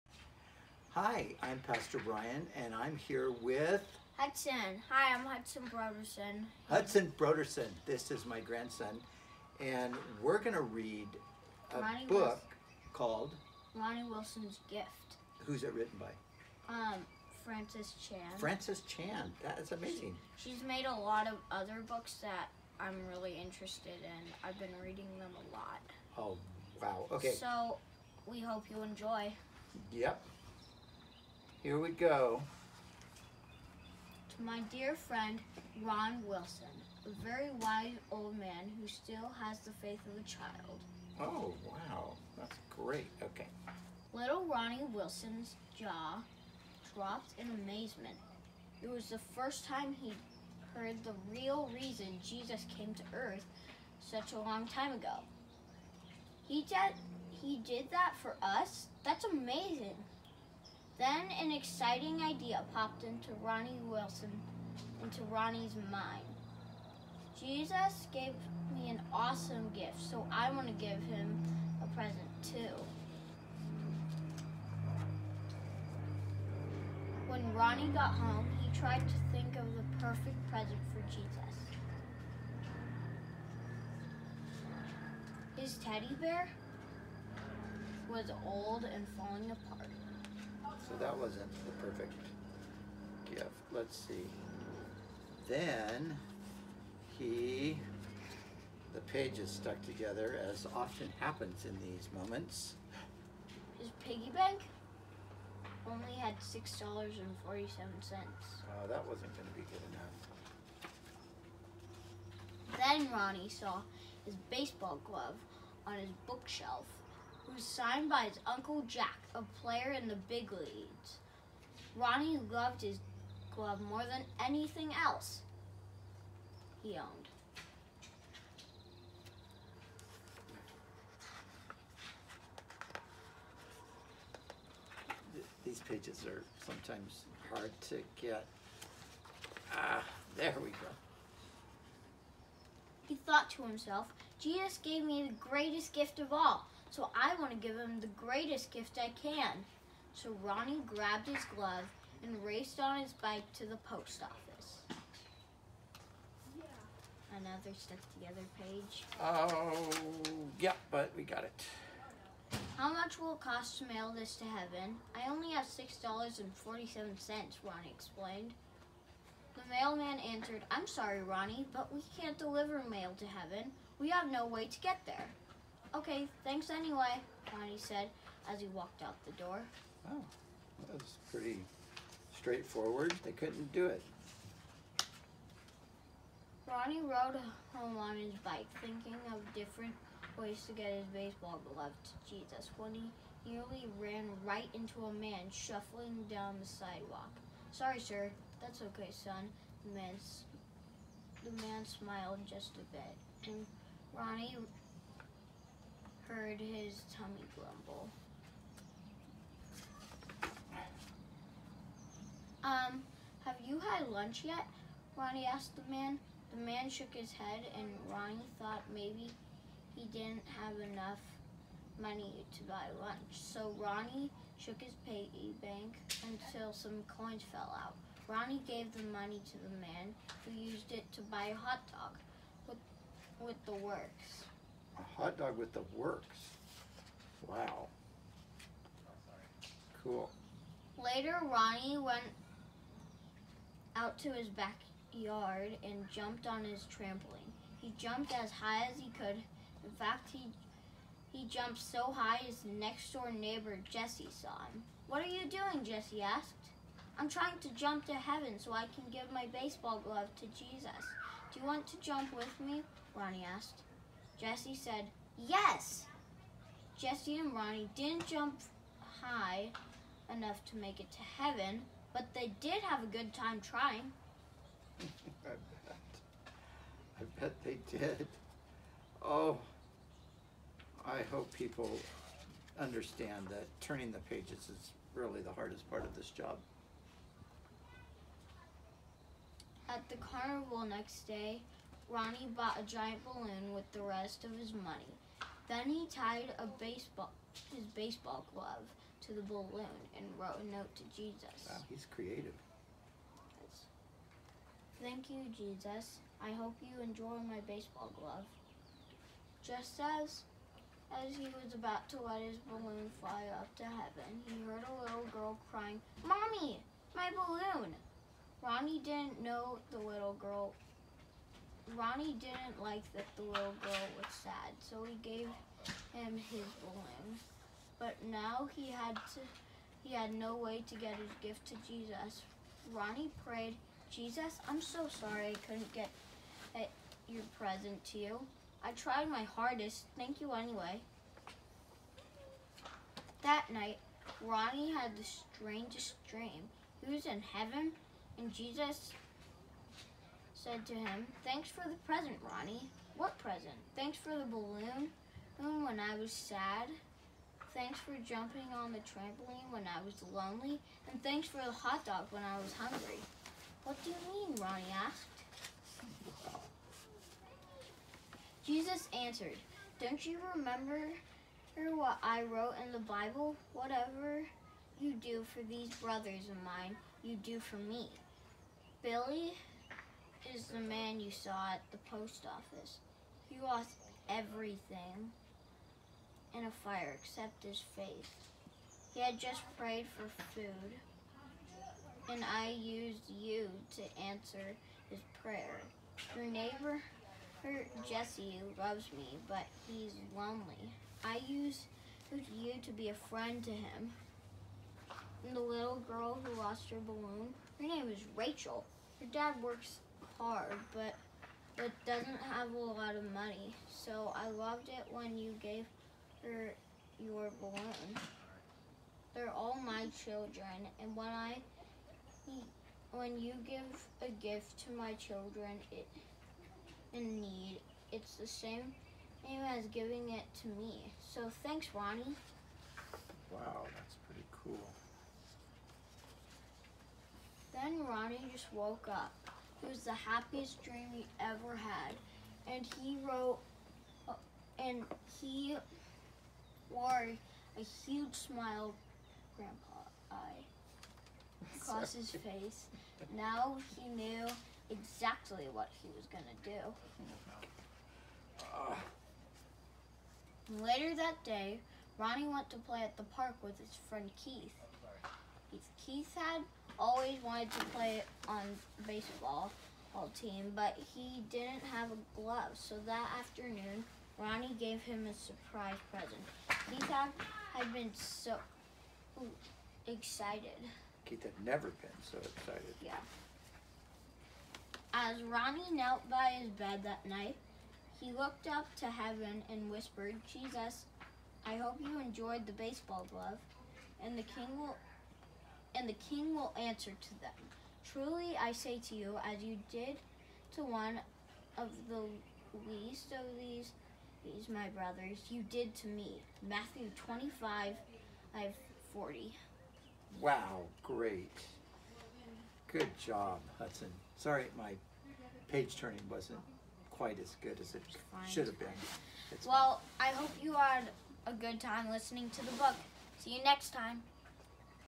Story Time: Ronnie Wilson's Gift
Join Pastor Brian Brodersen and his grandson, Hudson, as they read "Ronnie Wilson's Gift" By Francis Chan.